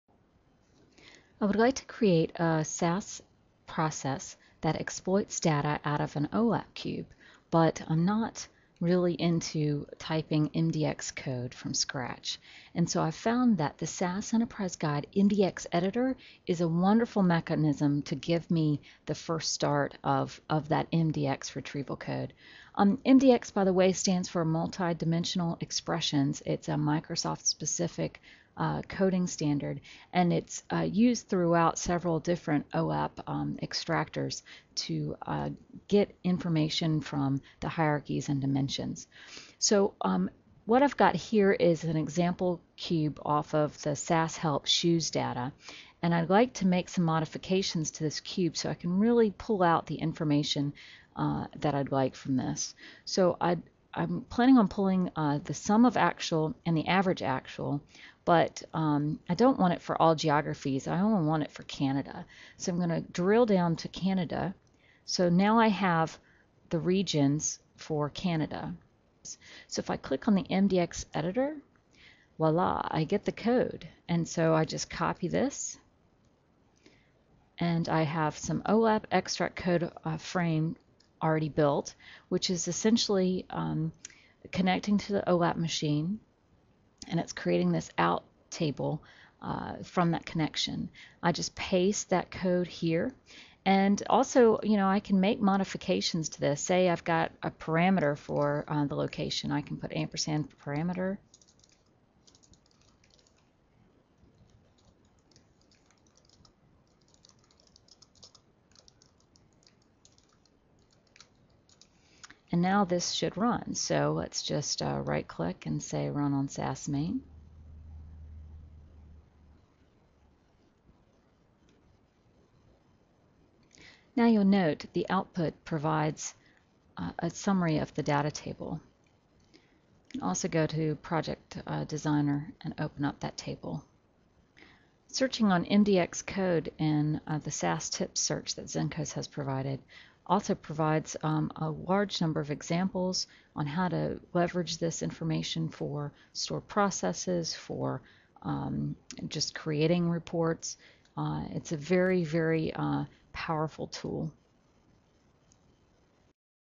Exploiting the MDX Editor in EG
Let SAS Enterprise Guide create the OLAP MDX code for you to use in SAS Stored Processes or other reporting!